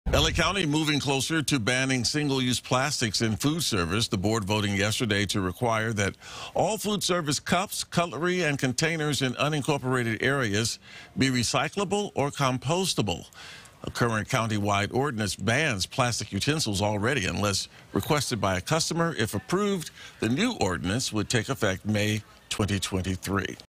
LA County moves closer to single-use plastic ban in food service
Hoping to cut down on the use of single-use plastics, the Board of Supervisors Tuesday gave preliminary approval to an ordinance requiring that all food-service containers, cups, dishes and cutlery distributed by restaurants and food facilities in unincorporated areas of Los Angeles County be recyclable or compostable.